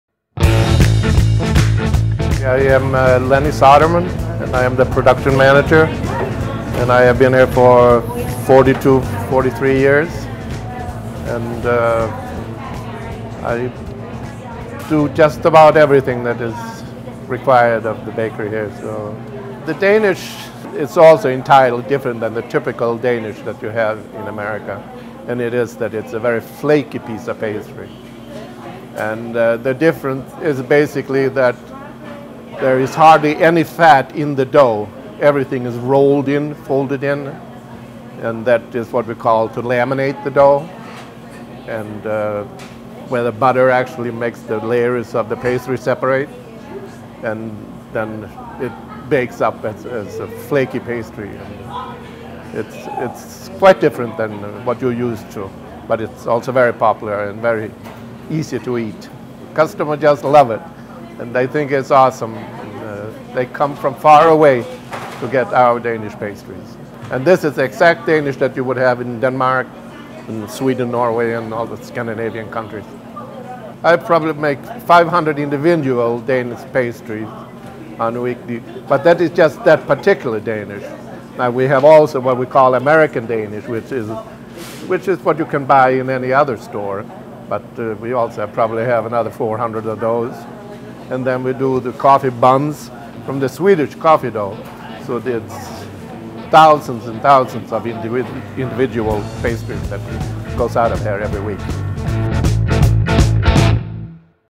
Danish Pastry
Crown Bakery shares breakfast ideas on the popularity of Danish pastries.